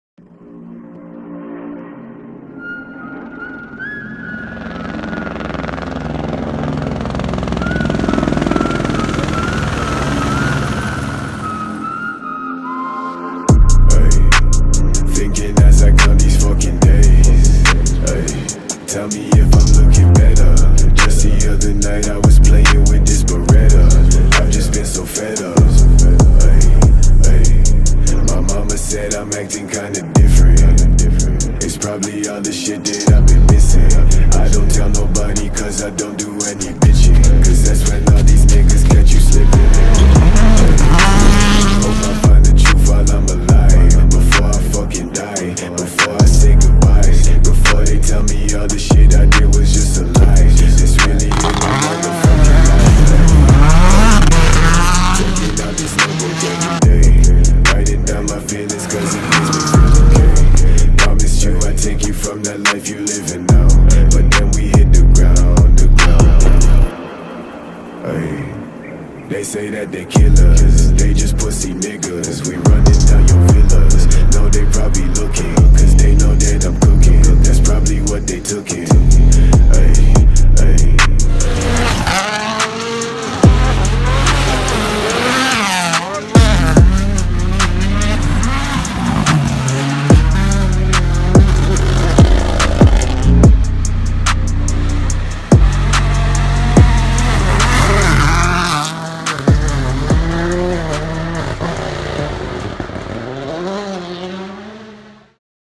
"ΤΟ ΒΟΥΝΟ ΘΕΩΝ" - "MOUNTAIN OF THE GODS" | Cinematic Video
WRC Rally Acropolis Greece, September 2022
I shot this entirely with an iphone 7 plus without any camera equipment just for fun. Enjoy!

Shot with iphone 7 plus